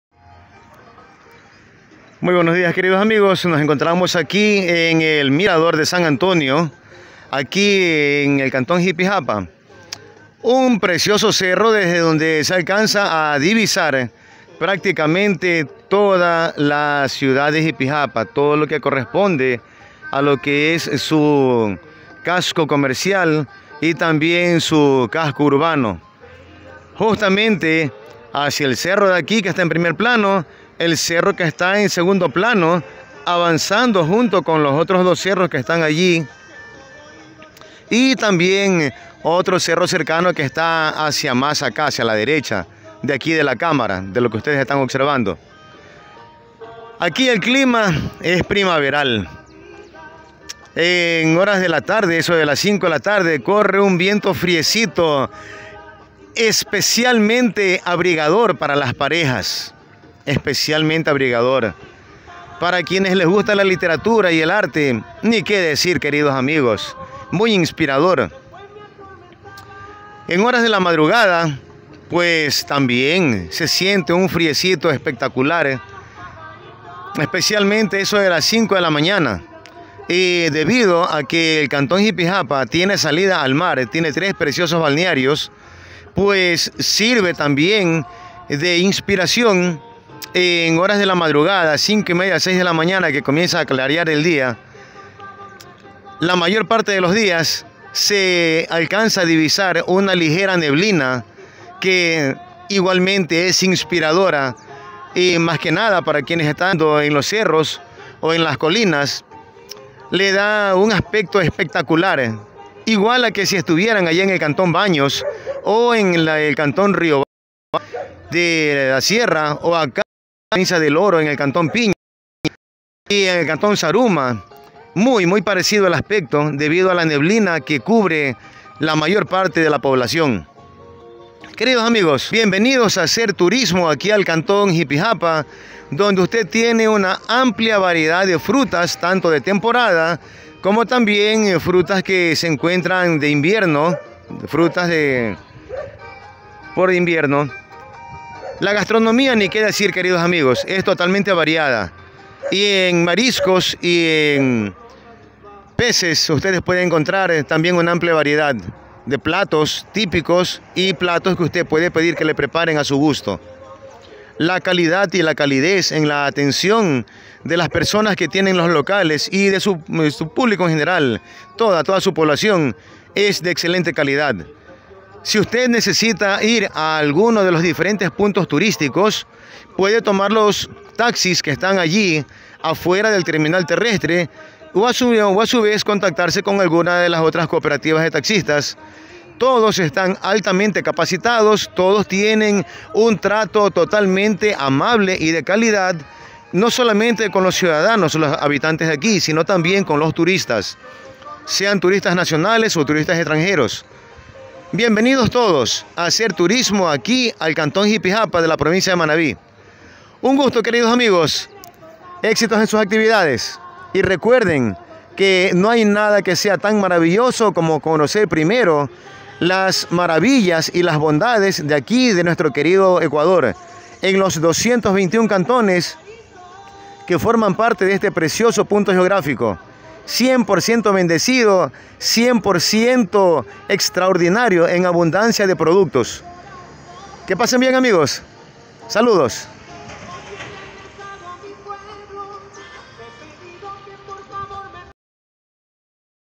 JIPIJAPA - MIRADOR de SAN ANTONIO.
Éste es uno de los 3 Miradores naturales que están al servicio de toda la ciudadanía Jipijapense  asi como tambien de TURISTAS NACIONALES Y EXTRANJEROS durante Todo el Año. Todos los Taxistas  están bien informados y por un valor MUY CONVENIENTE LO LLEVAN A CONOCER LOS 3 MIRADORES.